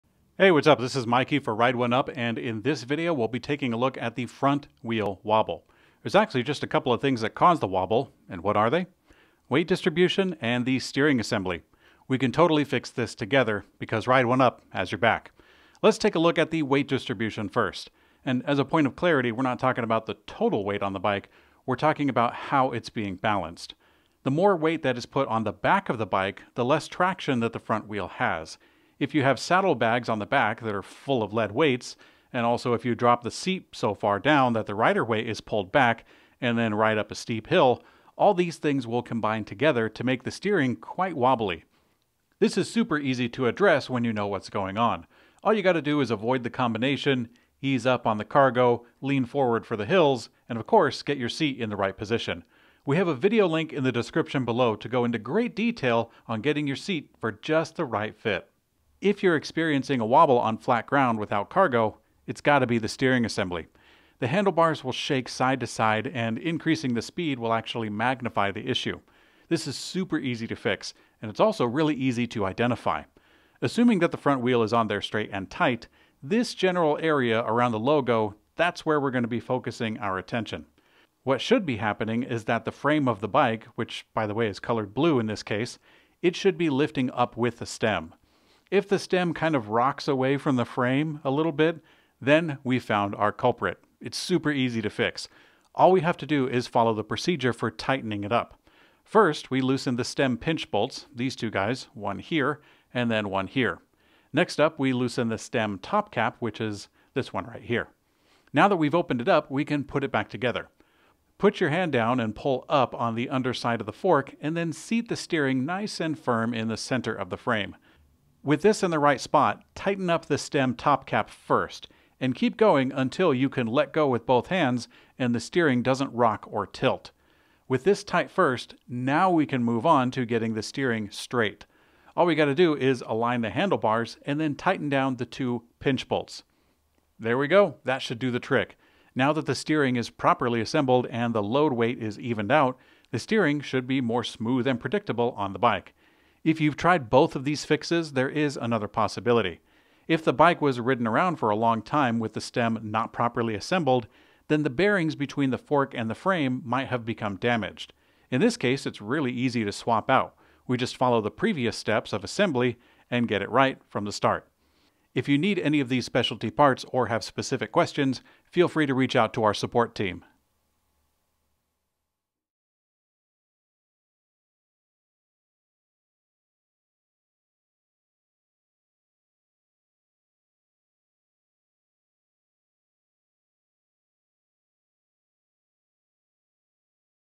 How to Fix Bike Wobble on the Front Wheel
Let's Fix the Front Wheel Wobble

0:00 Intro and Balance Wobble
0:43 Fix Balance Wobble
0:59 Steering Wobble
1:21 Verify the Steering Problem
1:35 Fixing Steering Wobble
2:26 Busted Bearings